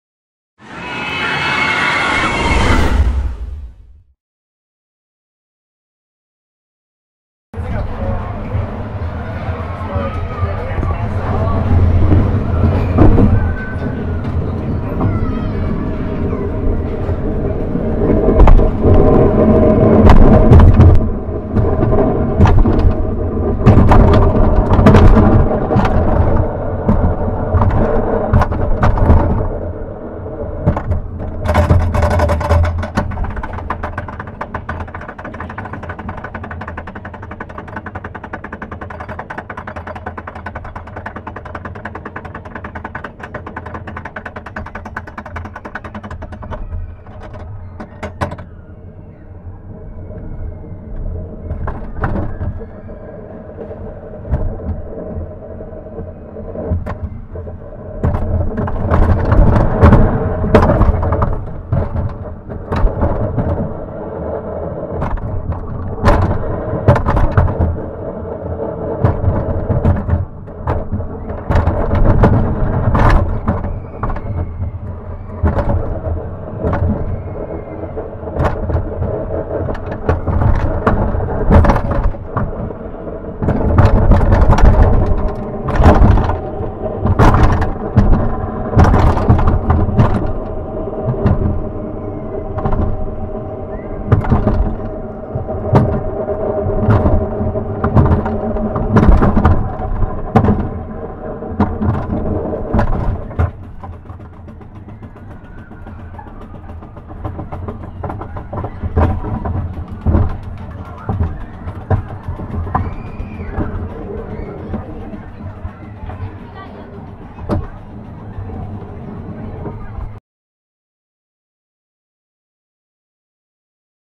Bobsleds front seat on-ride HD POV Seabreeze
Bobsleds Originally opened in 1952 as a wooden terrain coaster called Junior Coaster, but in 1968 the top layer of the structure and tubular steel track was added, making it a steel coaster.
Seabreeze is located in Rochester, New York.